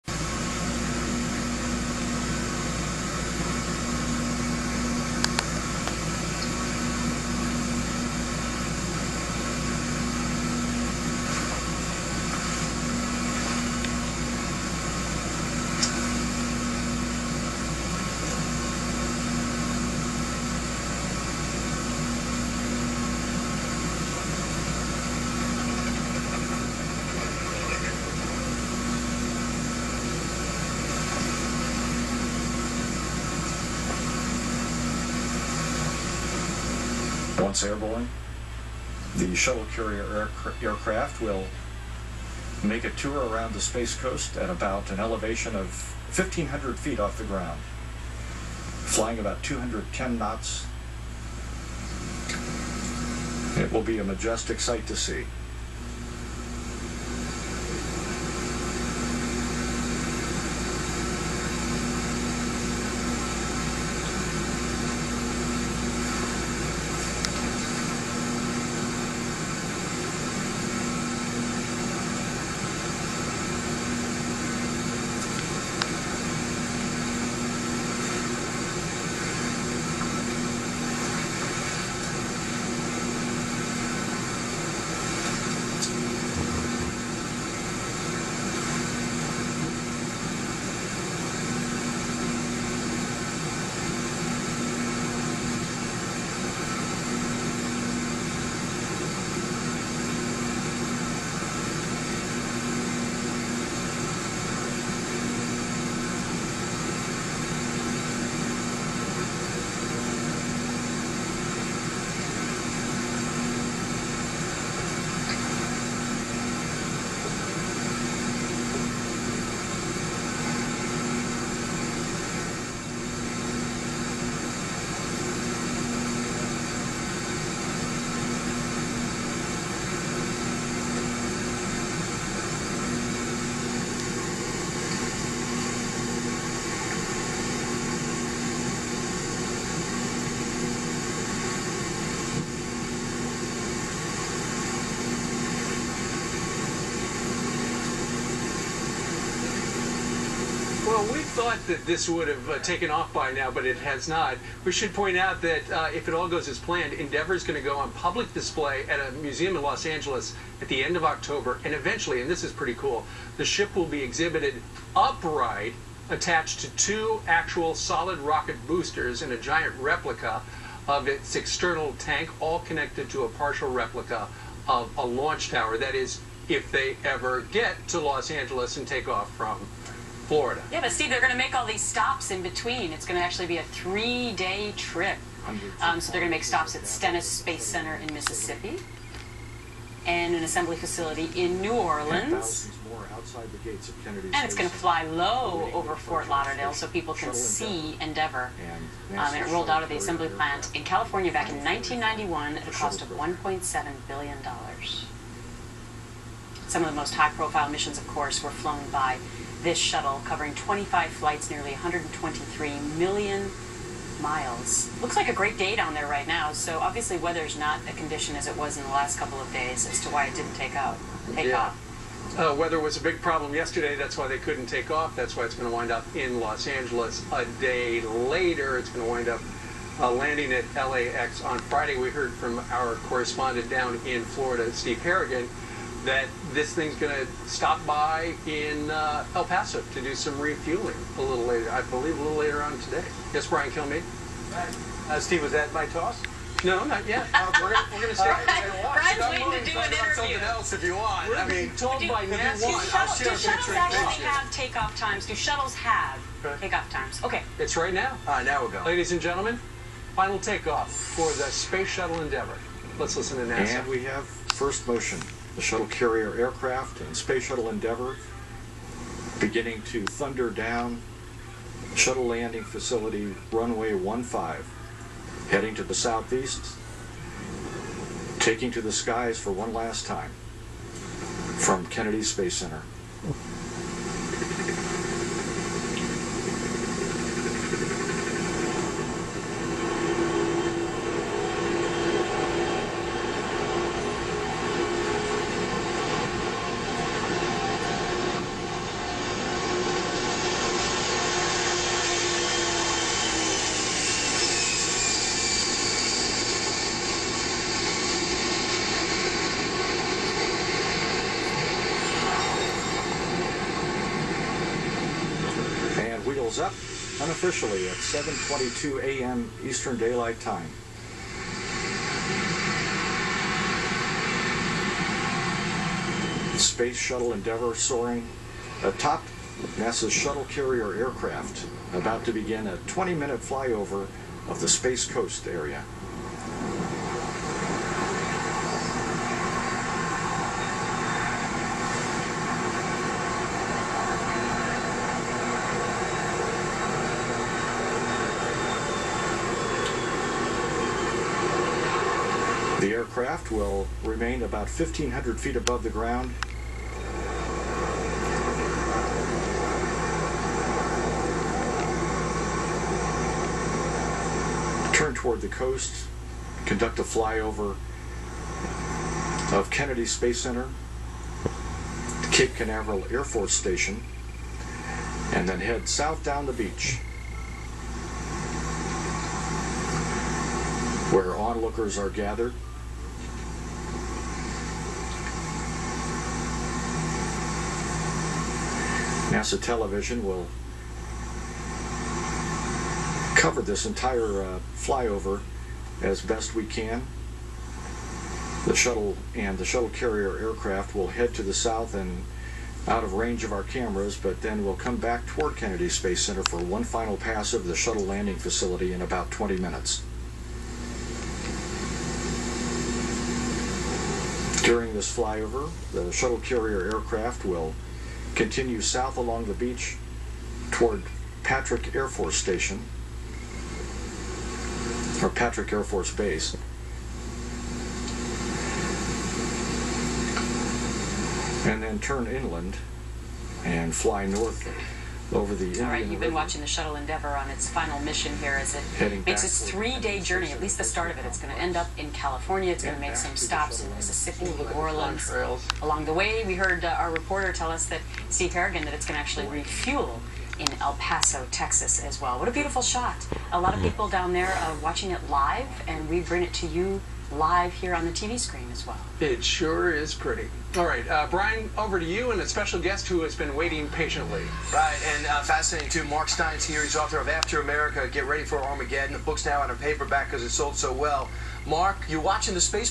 SPACE SHUTTLE blows up on tape
Space shuttel endevaer taking off 4 the last time.going to a museum.